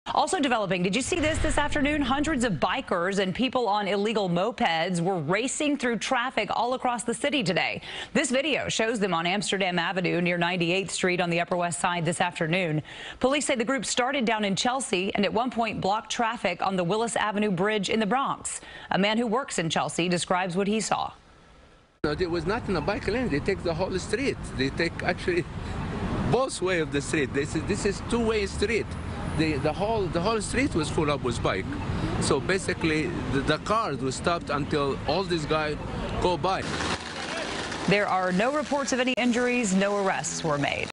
Bikers, illegal mopeds disrupt traffic in Manhattan and the Bronx
No arrests have been made, but local residents and people who work in the area say delays were prevalent in the impacted areas on Sunday afternoon. CBS2's Jessica Moore reports.